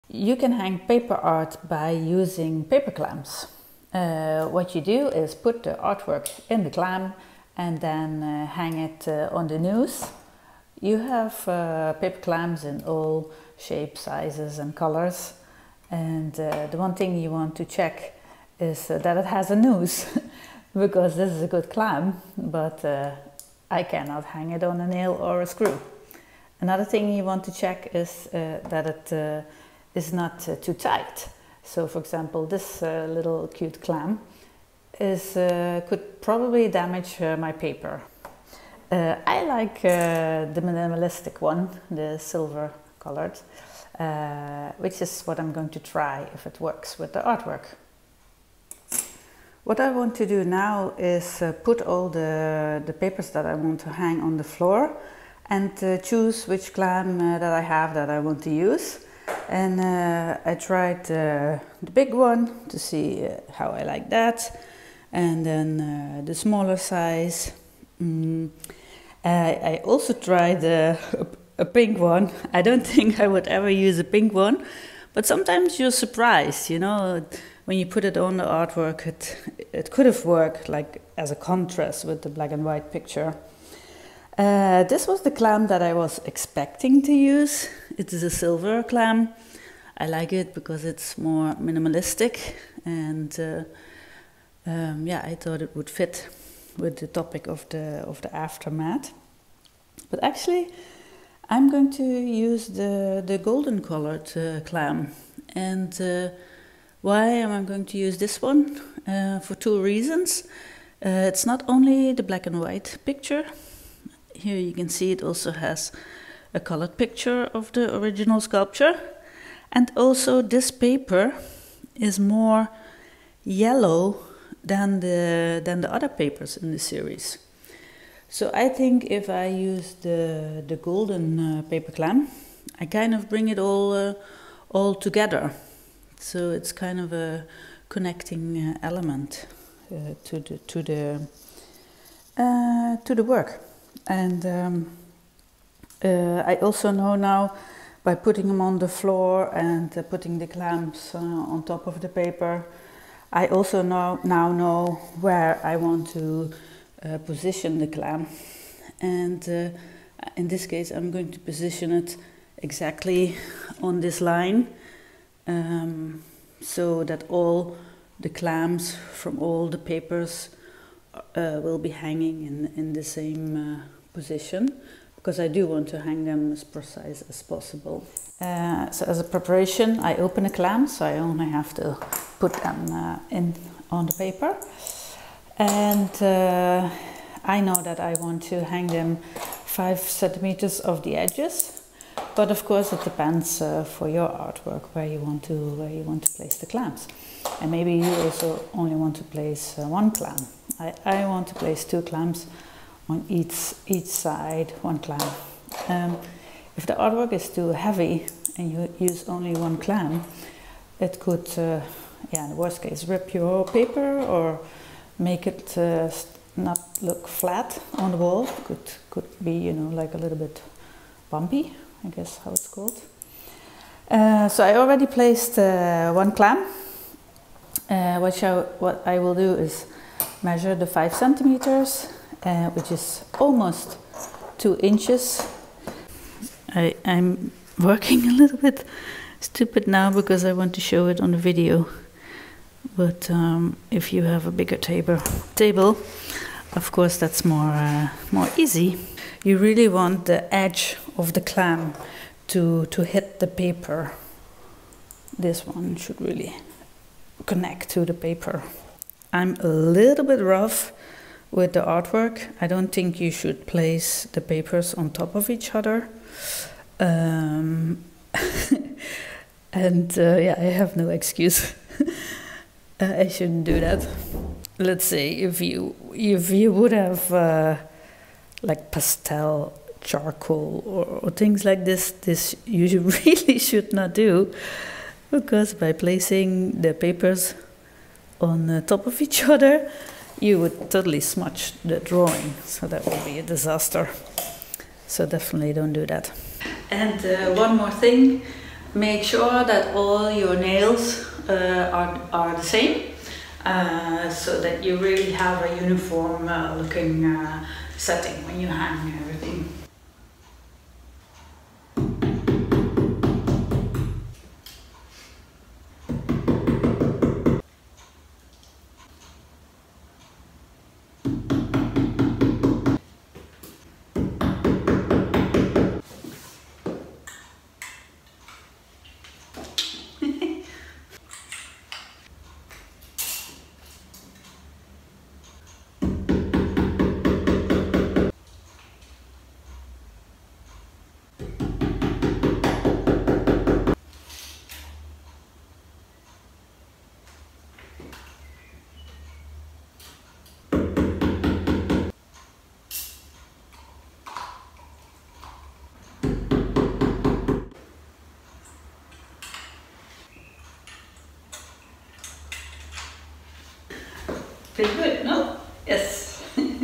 Hang Paper Art by using Paper Clamps
0:00  Choosing the Paper clamps
You can hang paper art by using paper clamps. What you do is put the artwork in the clamp and then hang it on the noose. You have paper clamps in all shapes, sizes, and colors. The one thing you want to check is that it has a noose. Because this is a good clamp. But I cannot hang it on a nail or screw. Another thing you want to check is that it is not too tight. So for example this little cute clamp could probably damage my paper. I like the minimalistic one,—the silver-colored. Which, is what I am going to try if it works with the artwork.

0:48  Papers on the Floor
What I want to do now is put all the papers that I want to hang on the floor. And choose which clamp that I have that I want to use. I tried the big one to see how I like that. And then the smaller size. I also tried a pink one. I don't think I would ever use a pink one. But sometimes you're surprised you know when you put it on the artwork it, it could have worked like as a contrast with the black and white picture. This was the clamp that I was expecting to use, it is a silver clamp. I like it because it's more minimalistic and yeah I thought it would fit with the topic of the aftermath. But actually, I'm going to use the golden-colored clamp. And why am I going to use this one? For two reasons: It's not only the black and white picture—here you can see it also has a color picture of the original sculpture. And also this paper is more yellow than the other papers in the series. So I think if I use the golden paper clamp I kind of bring it all together. So it's kind of a connecting element to the artwork.
And I also know now by putting them on the floor and putting the clamps on top of the paper I also know now know where I want to position the clamp. And in this case, I'm going to position it exactly on this line. So that all the clamps, from all the papers, will be hanging in the same position. Because I do want to hang them as precise as possible.

2:48  Connecting the Paper Clamps
So as a preparation I open the clamps, so I only have to clip them on the paper. And I know that I want to hang the paper clamps five centimeters from the edges. But of course, it depends on your artwork where you want to place the clamps. And maybe you also only want to place one clamp. I want to place two clamps, on each side one clamp. And if the artwork is too heavy, and you only used one clamp, it could, in the worst case, rip your paper. Or make it not look flat on the wall. It could be, you know like, a little bit bumpy—I guess how it's called. So I already placed one clamp. What I will do is measure the five centimeters, which is almost two inches.
I'm working a little bit stupid now because I want to show it in the video. But if you have a bigger table of course that is more easy. You really want the edge of the clamp to hit the paper. This one should really connect to the paper. I'm a little bit rough with the artwork. I don't think you should place the papers on top of each other. I have no excuse, I shouldn't do this. Let's say if you would have used pastel, charcoal, or things like this, this you really should not do. Because by placing the papers on top of each other, you would totally smudge the drawing, so that would be a disaster. So definitely don't do that.

4:56  Hanging the Work
And one more thing, make sure that all your nails are the same. So that you really have a uniform-looking setting, when you hang everything.

Pretty good no? Yes.